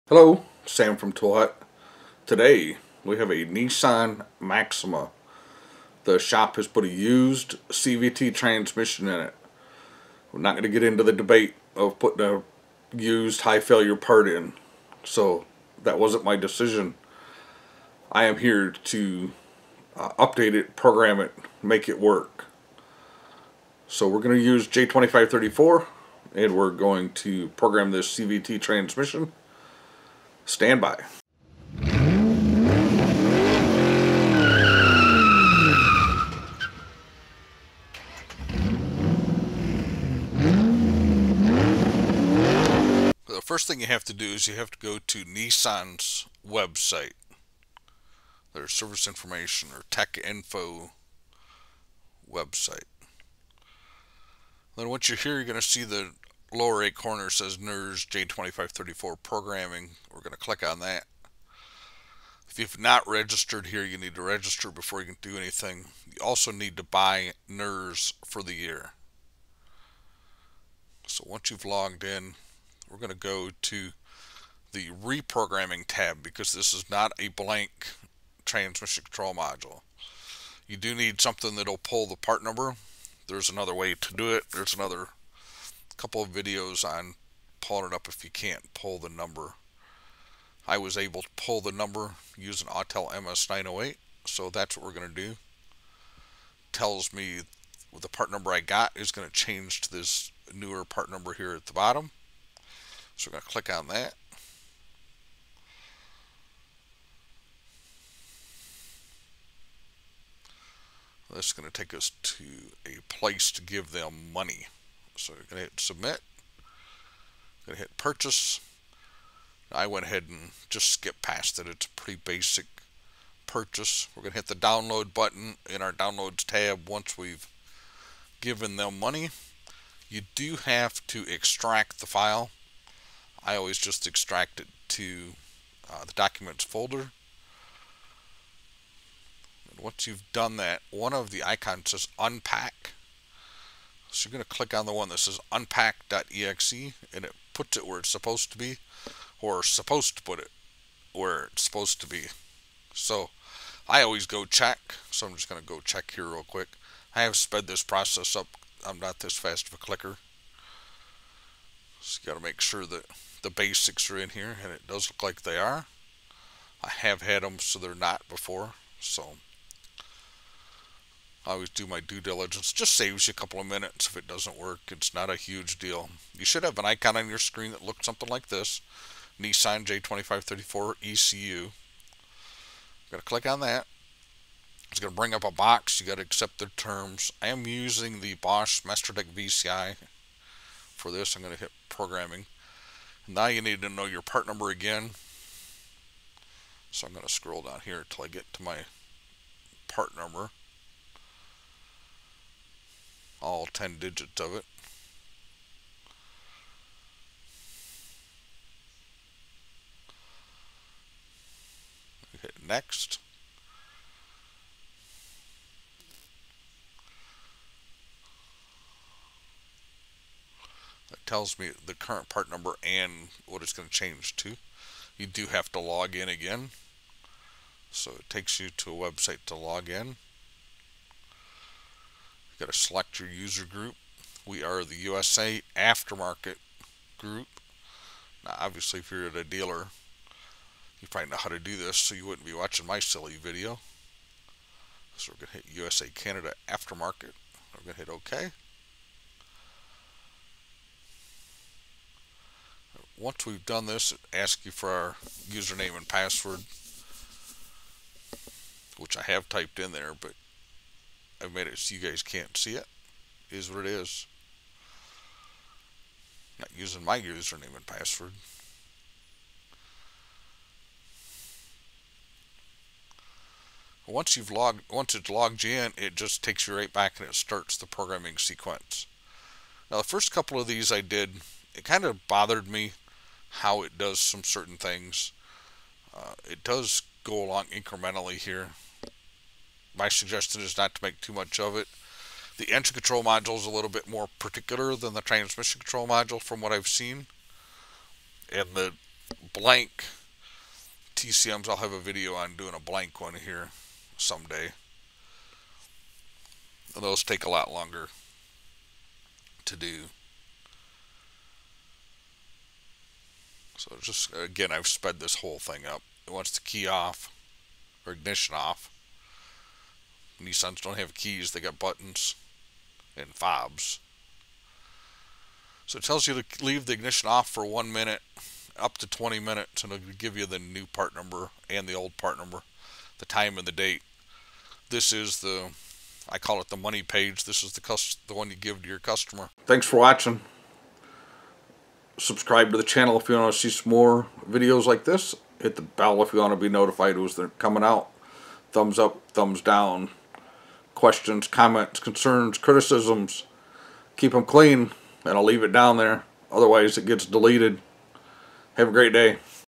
Nissan Maxima TCM J2534 NERS
Watch as we update a used CVT TCM using J2534 and NERS.

Want a ToolHutUSA sticker send $5 USD specify it is for a sticker. Outside US please add some extra for additional postage.